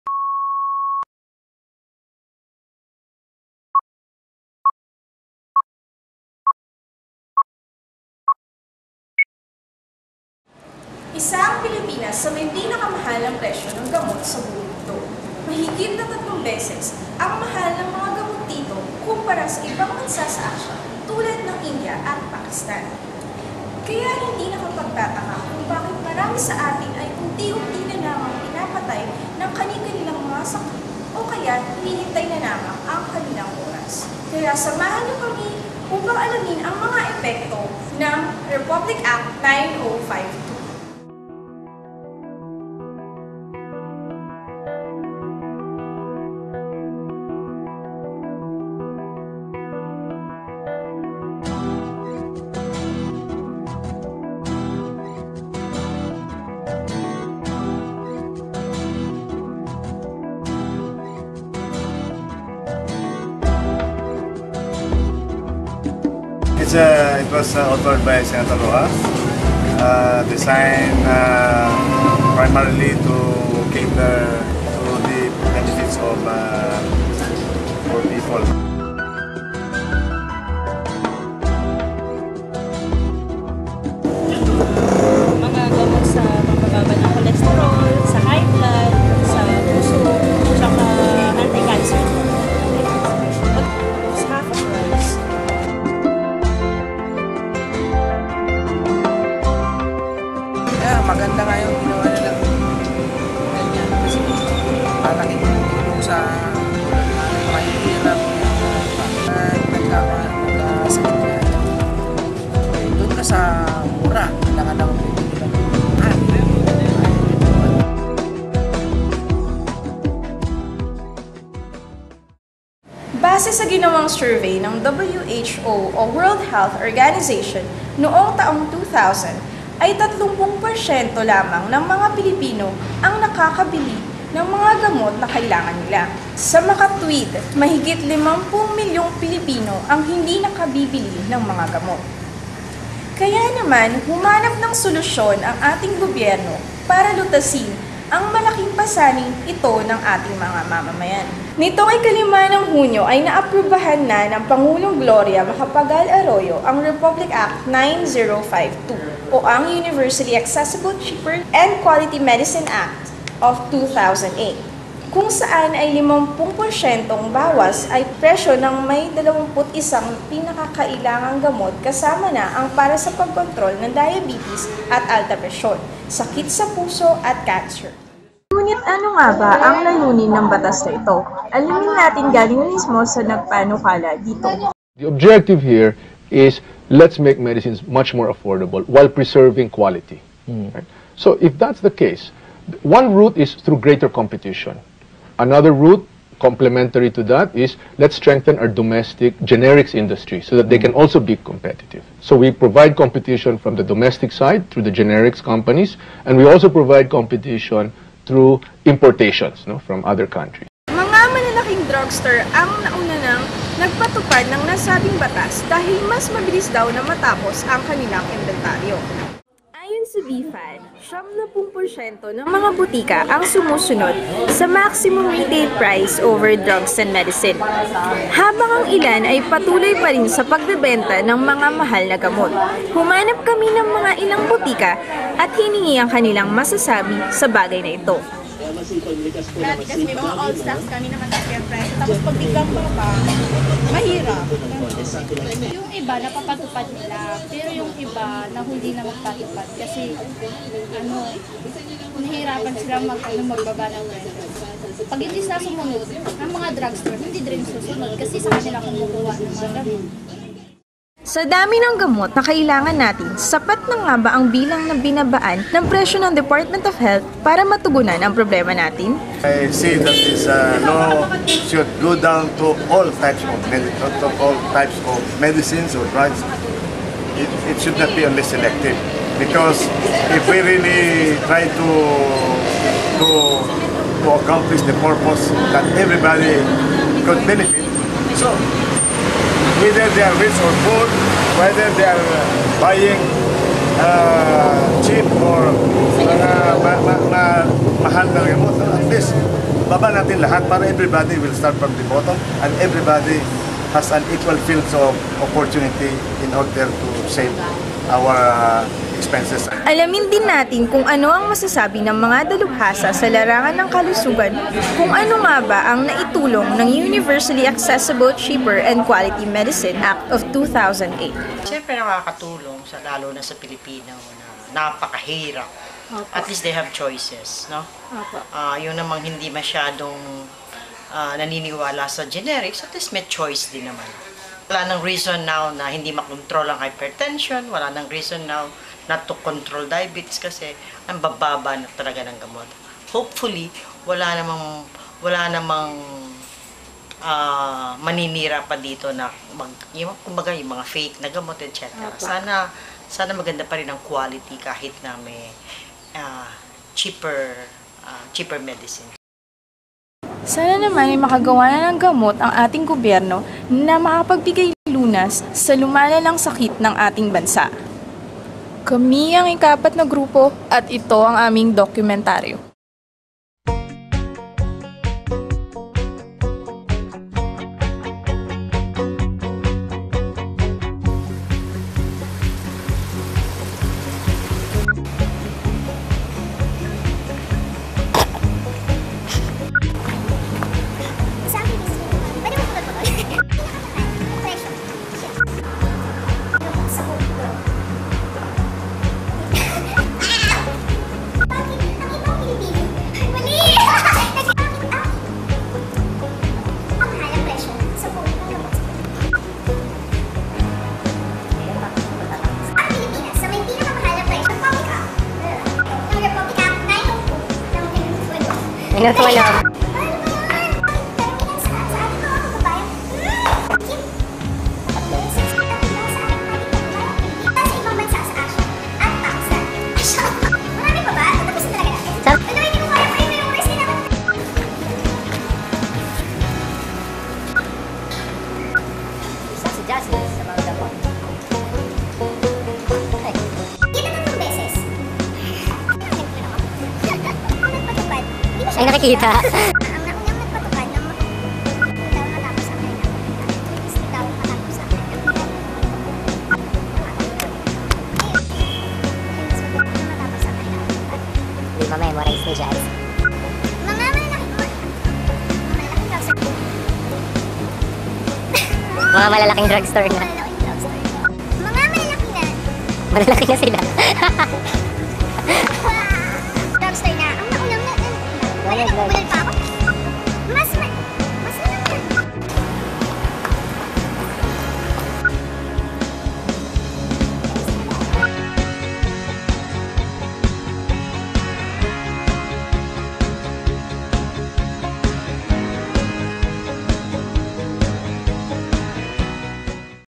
Republic Act 9052 - A Short Documentary Film
This short film is about the effects of the "Cheaper Medicines Act of 2008" in the Philippines.

I am proud to share with you our group's joy and hardwork.
The achievements of this film has brought pride and honour to our group. It has collected quite a handful of mentions in the CHD Documentary Film Awards last October 26, 2009 at FEU-NRMF:
1. Nominated Best Unique Film
2. Nominated Best Health Advocate Film
3. Nominated Best Researched Film
4. Nominated Best Cinematography
5. Nominated Most Informative Film
6. Won the award of Best Cinematography

Thank you for all the support! To God be the Glory!